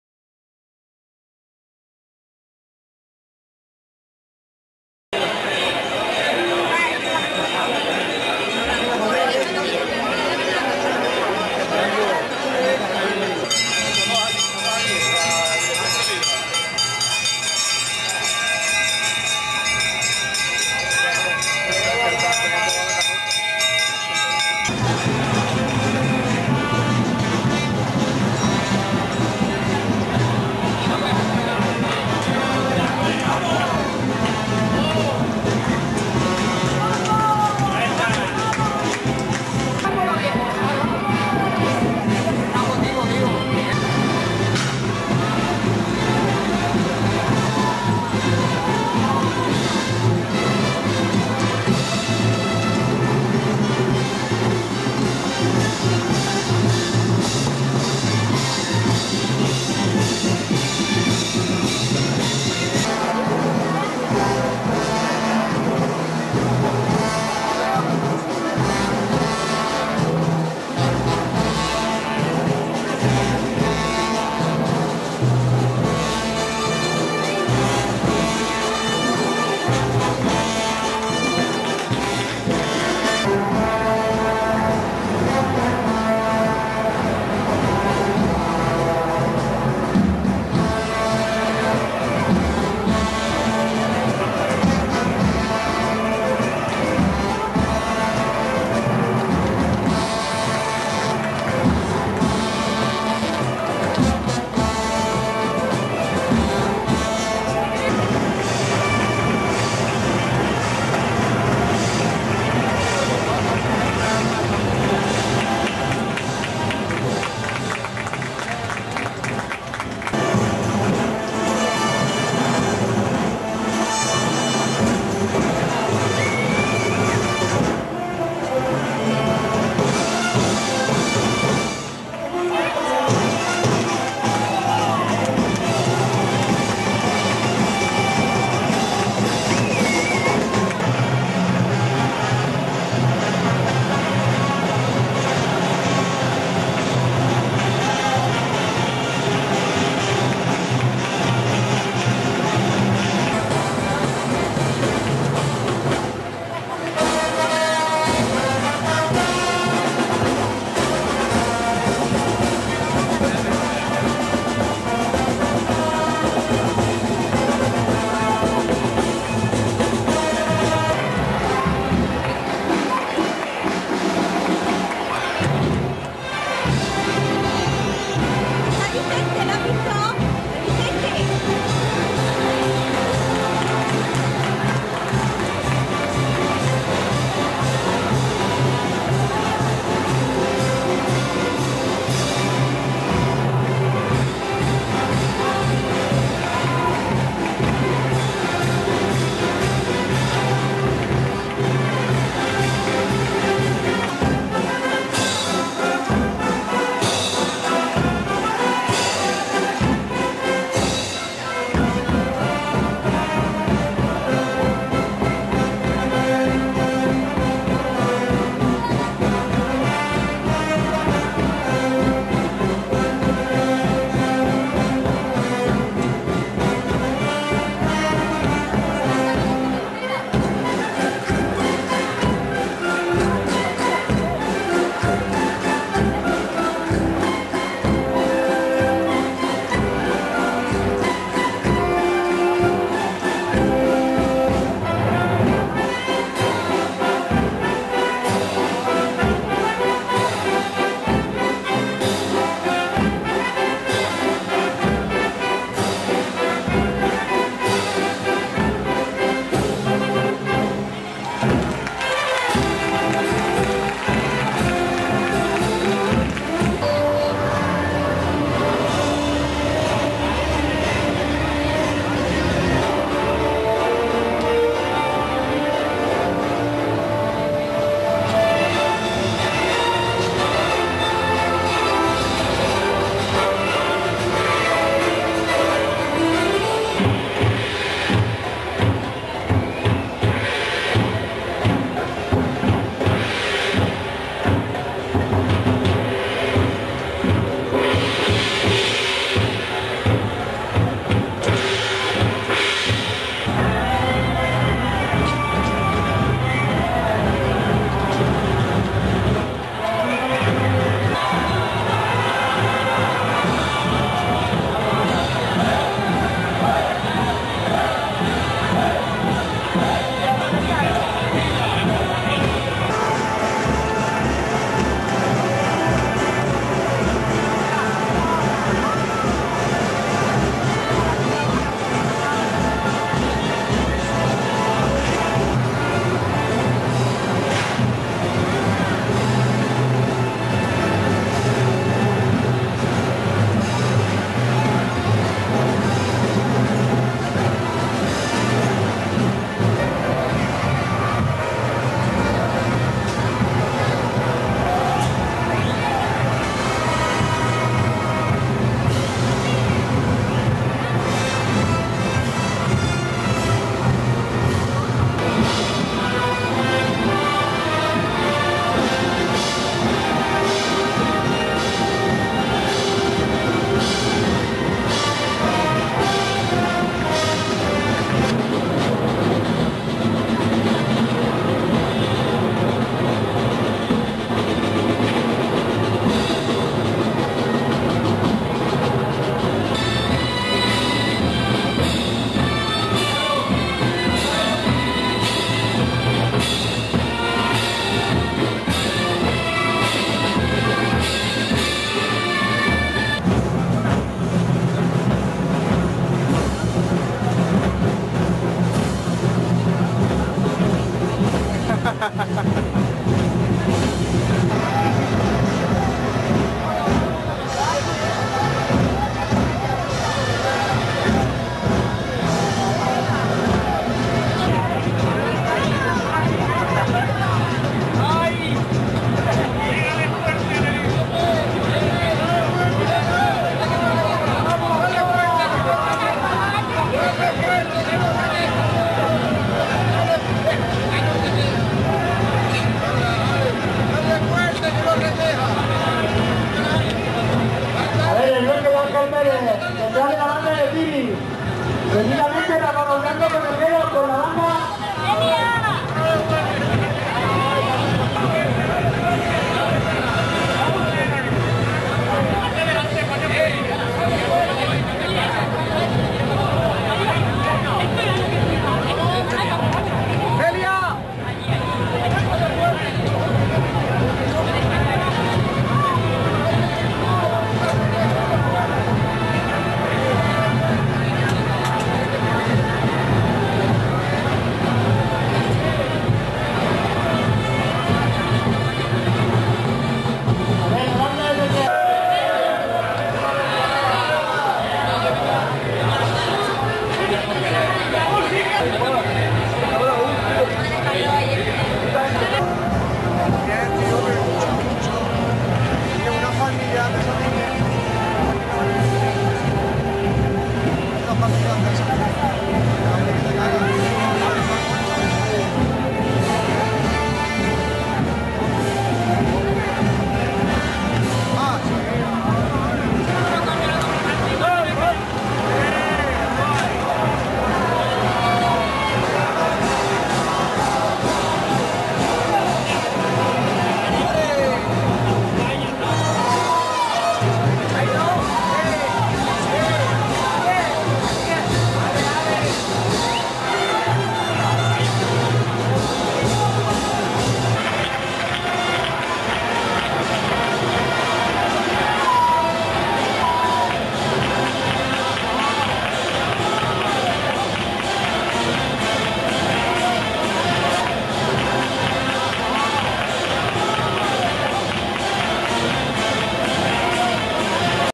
Villena Dia 6 Cabalgata
De todos los desfiles el más bonito de ver es sin lugar a dudas el de la Cabalgata. Donde los focos hacen brillar los metales de los trajes.

Lo malo que tiene es que empieza a las 9 de la noche y termina alrededor de las 4 mañana. Es decir que es un disfrute solo para los más sacrificiados.
Algunas escuadras especiales se dotan de los llamados "boatos", pequeñas representaciones que dan más luz y colorido a la fiesta. A mi particularmente el que más me llamó la atención fue el de los Moros Realistas donde se representaba la campanas que el caudillo moro Almanzor robó de Santiago de Compostela.